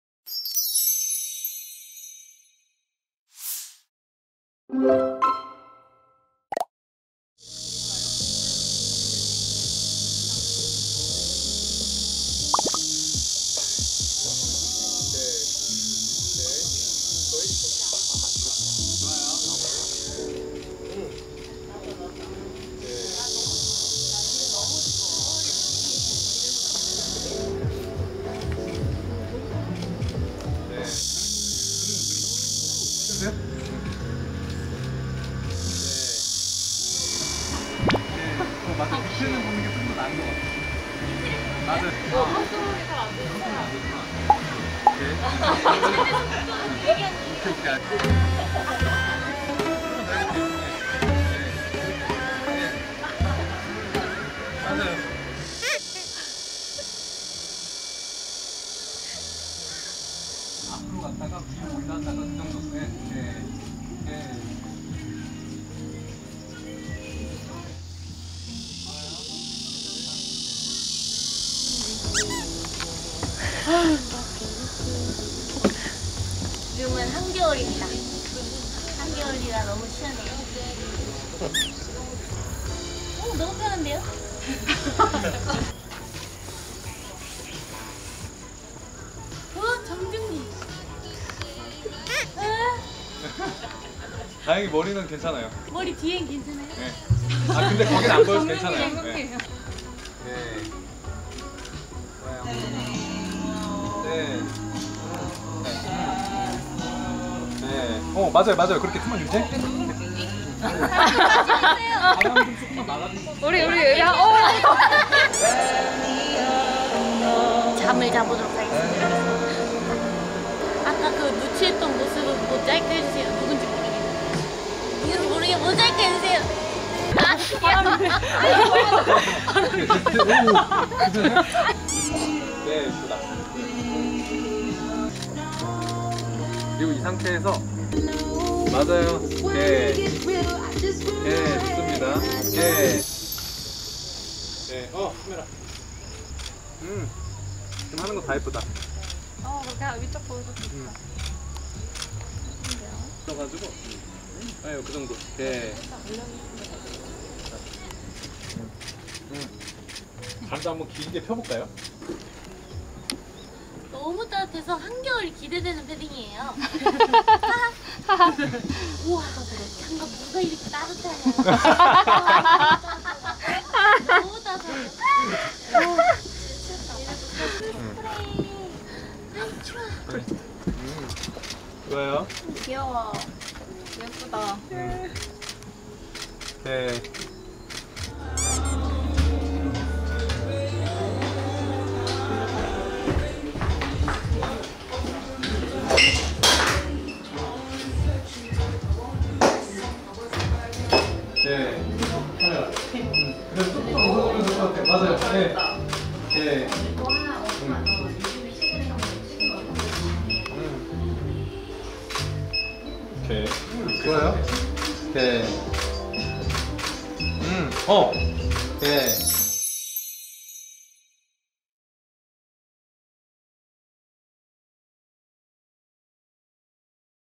[CHUU-ing] 여름에 야외에서 롱패딩 입고 촬영하다가 대왕 홍시에게 잡아먹힌 이야기 | 아키클래식 2023 FW 촬영 비하인드
✦ More information about CHUU